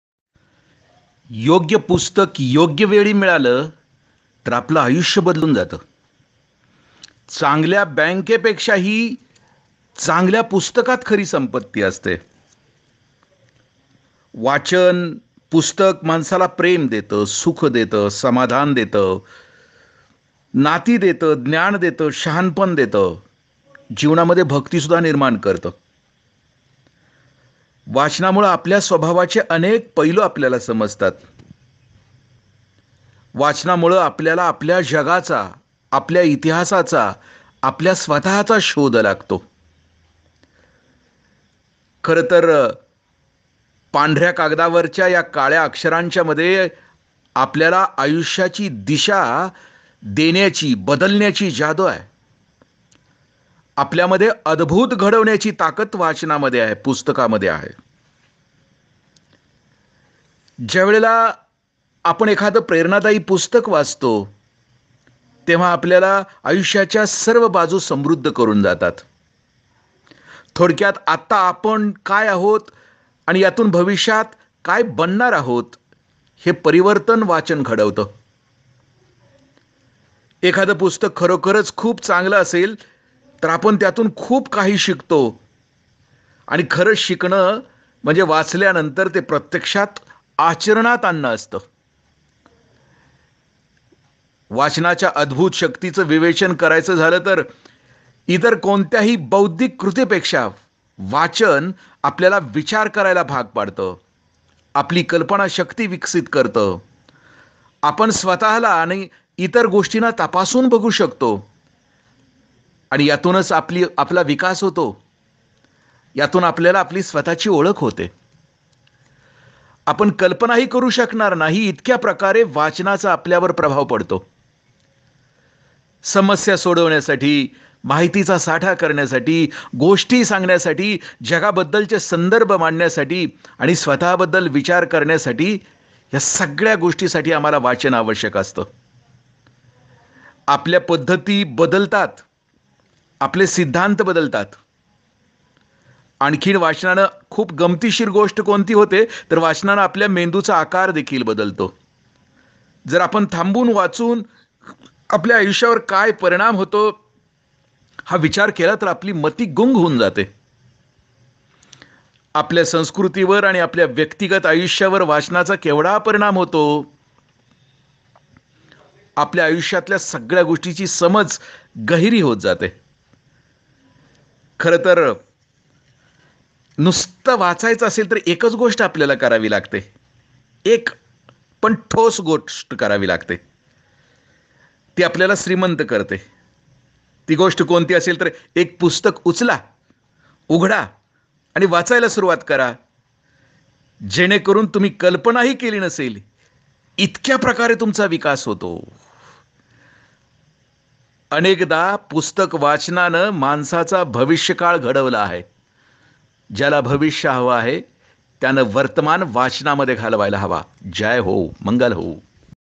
योग्य पुस्तक, योग्य वेळी मिळाले तर! आपलं आयुष्य बदलुन जात - आदरणीय इंद्रजीत देशमुख साहेब तथा काकाजी.
Vachankatta/indrajit deshmukh/ book thought